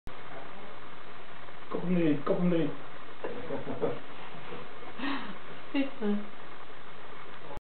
H kijkt voetbal en vraagt zich af waar het balletje blijft.MOV
Ragdoll kitten House, voetbal kijken, maar waar blijft dat balletje nou??